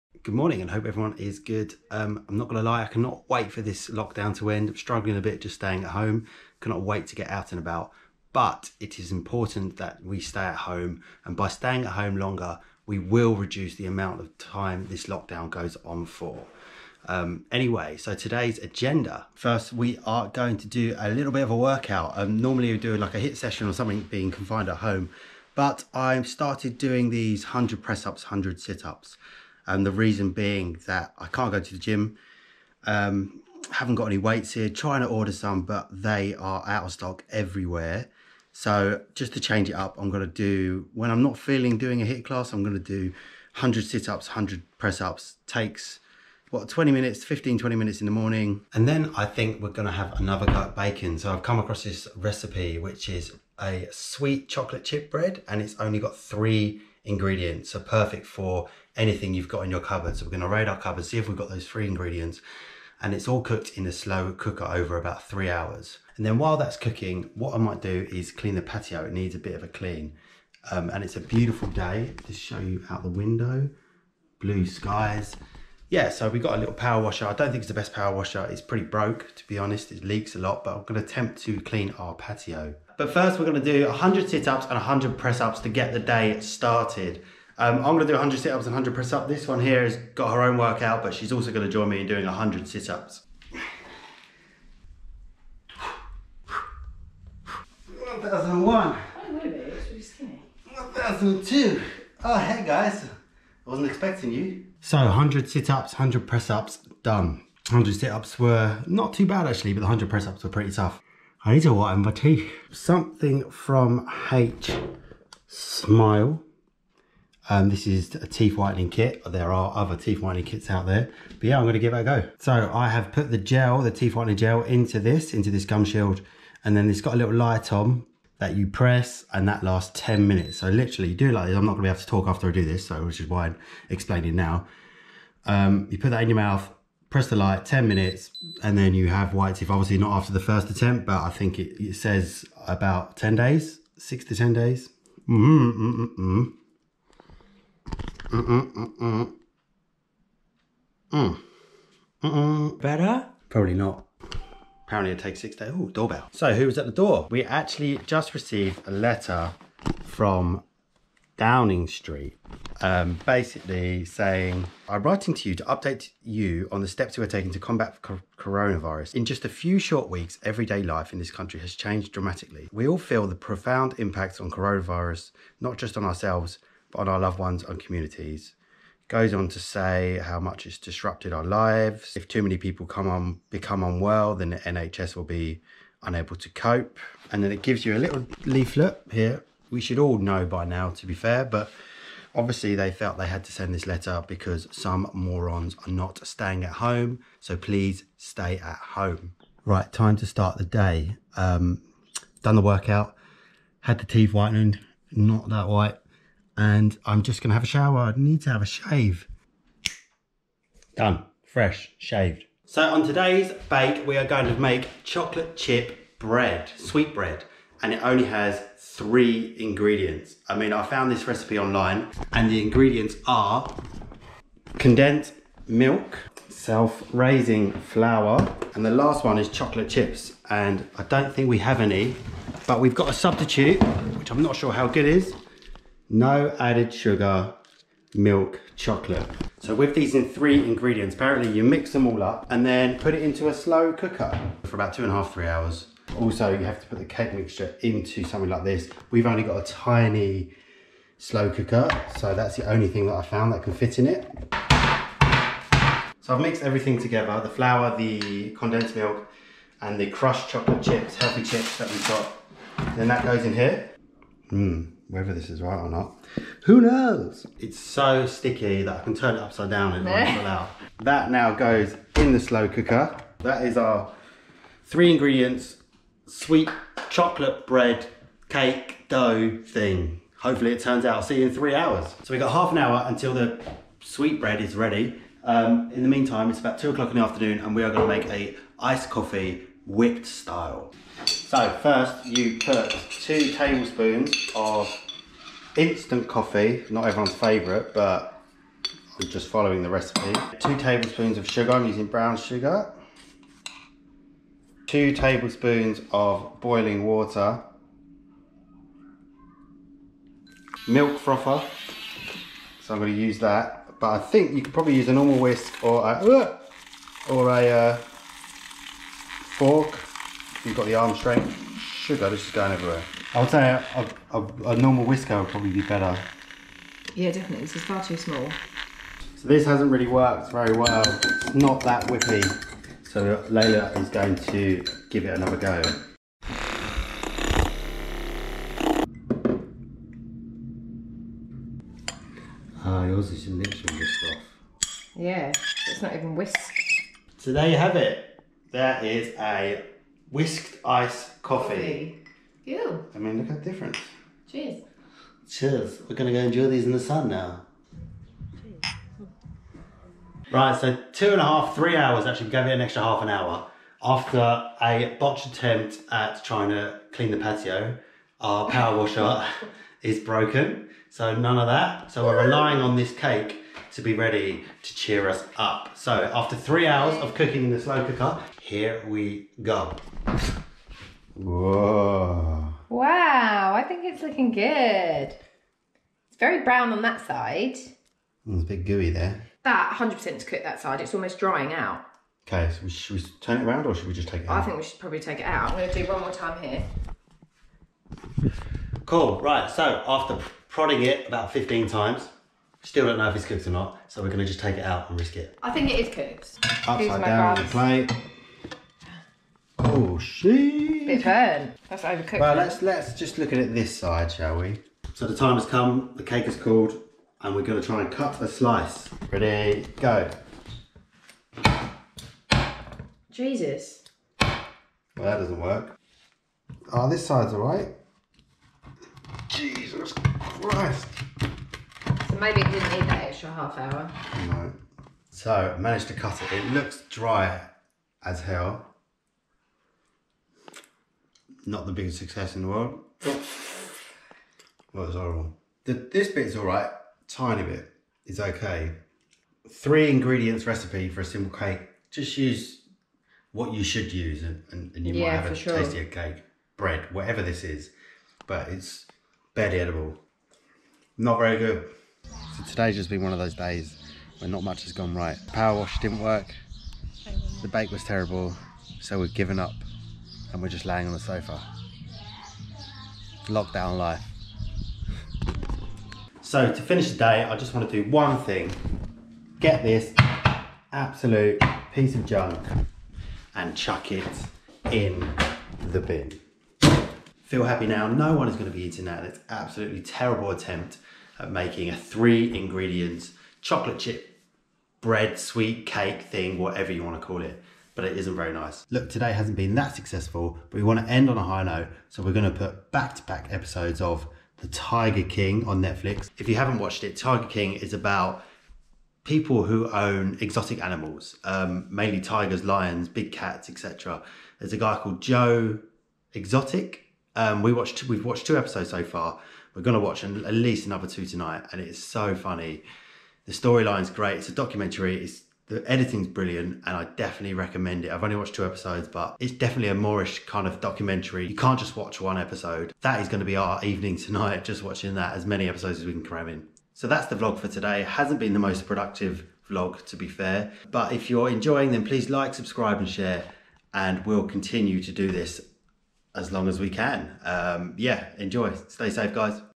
WHEN THINGS DON'T GO TO PLAN DURING LOCKDOWN!!
VLOG 7 - WHEN THINGS DON'T GO TO PLAN DURING LOCKDOWN!!

This day vlog did not go well. From trying to bake an easy cake, making a whipped coffee to planning to powerwash our patio. It was just one of those days!!

Oh well. We go again!